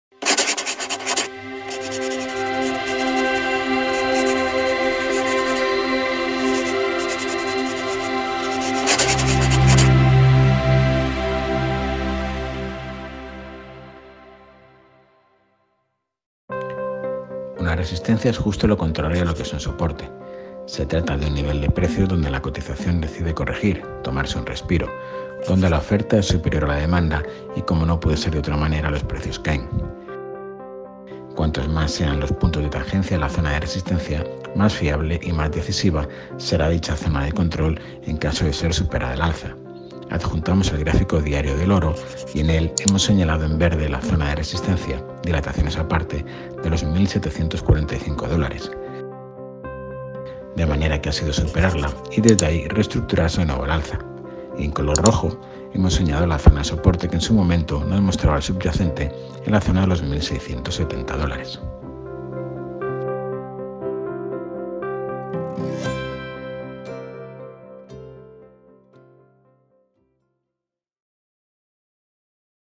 ✏️ Aprende con Bolsamanía 2 - ¿Qué es una resistencia?
Segunda lección de Aprende con Bolsamanía. Hoy veremos qué es una resistencia de la mano de nuestro maestro del análisis técnico, José María Rodríguez.

Una resistencia es justo lo contrario a lo que es un soporte. Se trata de un nivel de precios
donde la cotización decide corregir, tomarse un respiro. Donde la oferta es superior a la
demanda y como no puede ser de otra manera los precios caen.